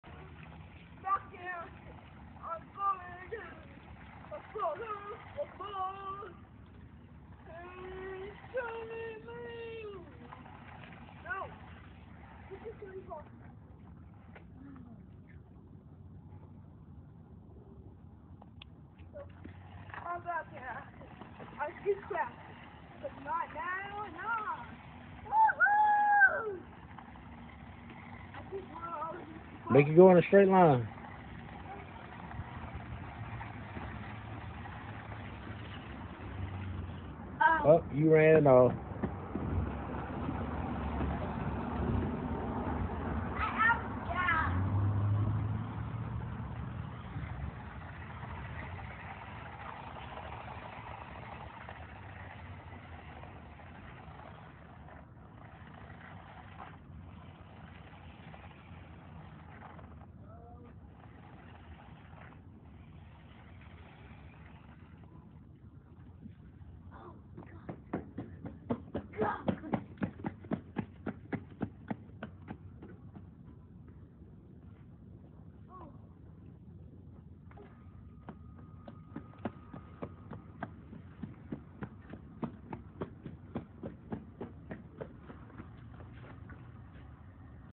Kayden coleslaw remote control car
Playing with his car at his Uma house